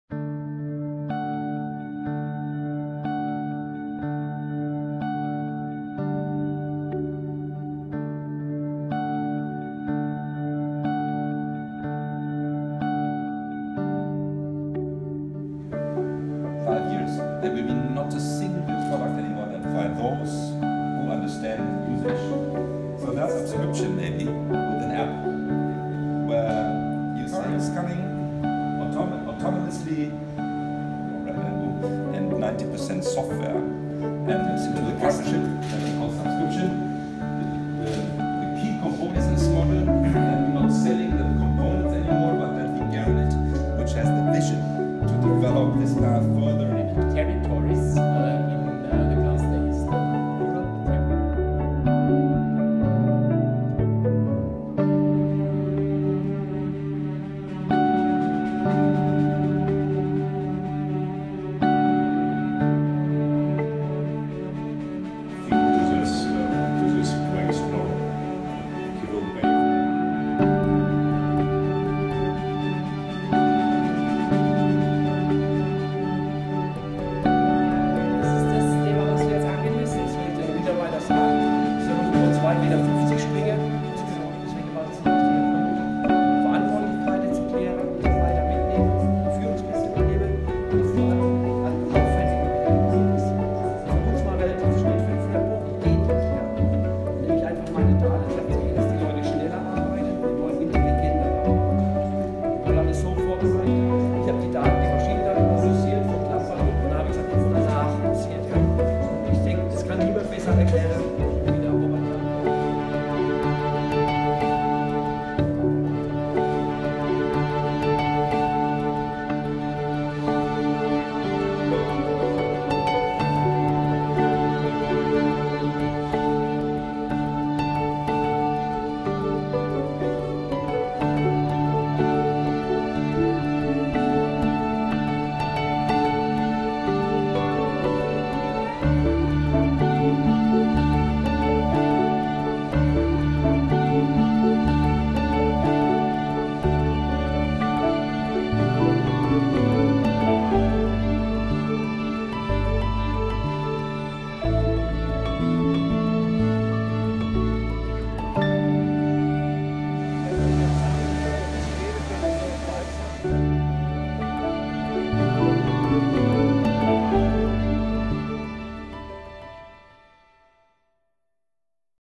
Impressions at Klampfer Druck 1st Subscription Media Event
Impressions from the 1st Subscription Media Event in Austria / Impressionen vom 1. Subscription Media Event in Österreich

The 1st Subscription Media Event at Klampfer Druck was supported by Heidelbergger Druckmaschinen AG. Chief Digital Officer Prof. Dr. Ulrich Hermann  and his team shared great insights, facts and learnings. And you know what: subsriptioneconomy works for innovative print biz entrepreneurs to make a good company even better and fit for the future.

Der 1. Subscription Media Event bei Klampfer Druck wurde von der Heidelberger Druckmaschinen AG unterstützt. Chief Digital Officer Prof. Dr. Ulrich Hermann und sein Team teilten großartige Einsichten, Fakten und Erkenntnisse. Und wissen Sie was? Die Subskriptionswirtschaft passt perfekt für innovative Print-Unternehmer, um ein gutes Unternehmen noch besser und zukunftsfähiger zu machen.